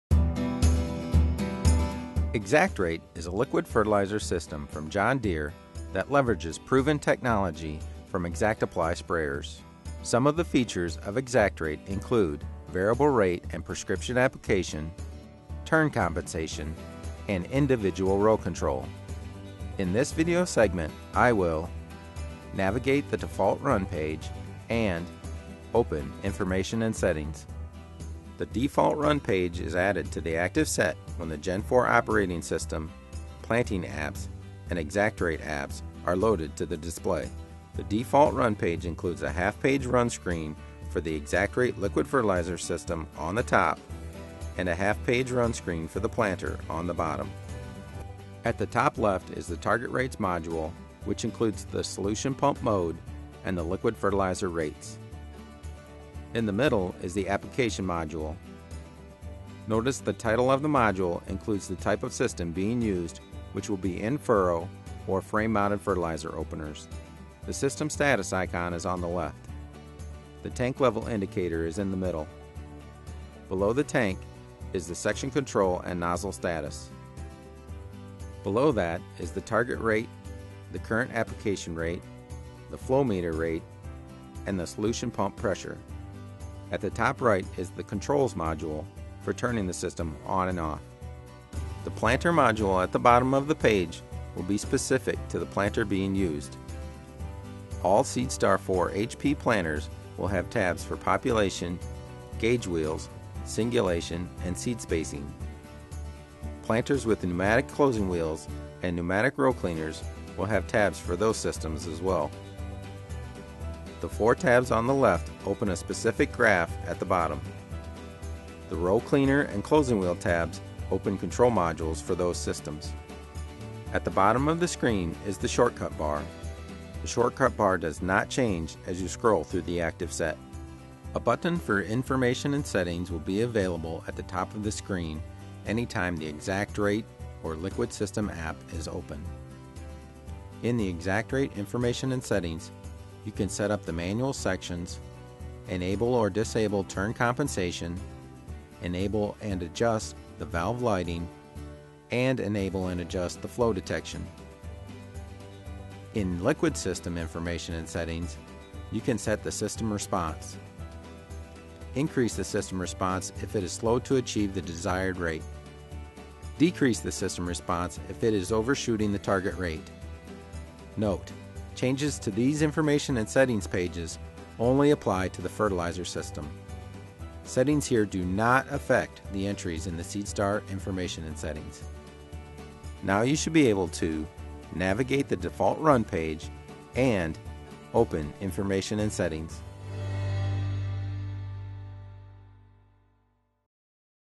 How To Use The Default Run Page | John Deere ExactRate™ Liquid Fertilizer System
ExactRate™ is a liquid fertilizer system from John Deere that leverages proven technology from ExactApply™ sprayers. Some of the features of ExactRate include variable rate and prescription application, turn compensation, and individual row control.

In this video segment we will navigate the default run page and open information and settings.

0:00 Default Run Page
0:47 Fertilizer Controls
1:31 Planter Controls
2:12 ExactRate Information and Settings
2:35 Liquid System Information and Settings